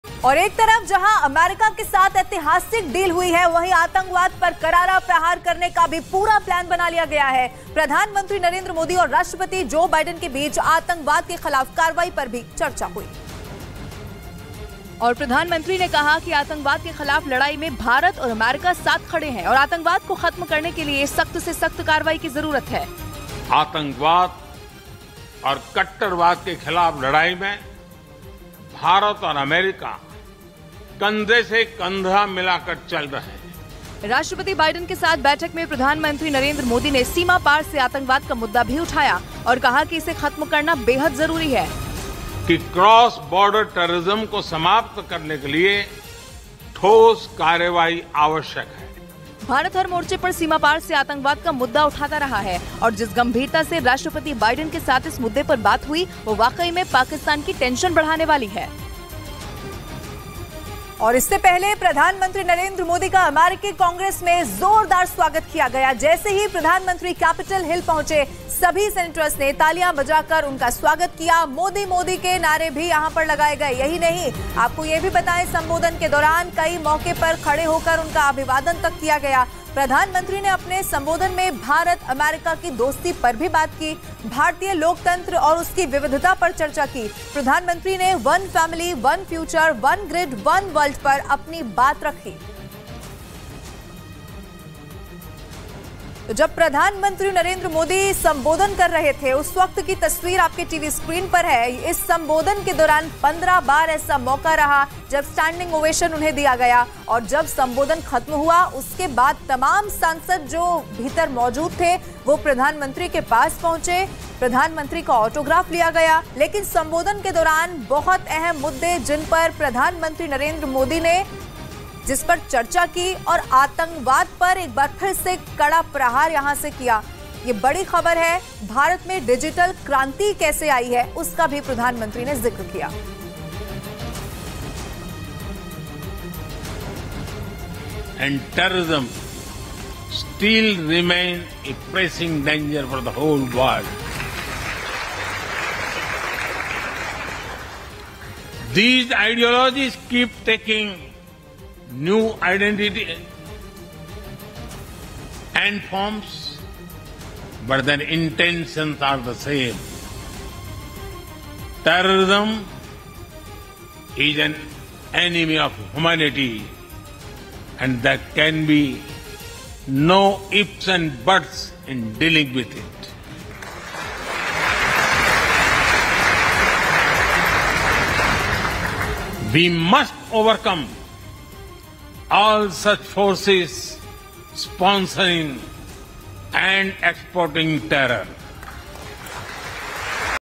आतंकवाद पर PM Modi ने ऐसा क्या कहा जिससे Pakistan की बढ़ गई टेंशन? | White House | News18 India
Terrorism was also discussed between PM Modi and Joe Biden, PM Modi said – India and America have stood together on terrorism. Also said that it is very important to end it.
पीएम मोदी और जो बाइडन के बीच आतंकवाद पर भी चर्चा हुर्ई, पीएम मोदी ने कहा- आतंकवाद पर भारत और अमेरिका साथ खड़े हुए हैं। साथ ही कहा इसे ख़त्म करना बहुत ज़रुरी है।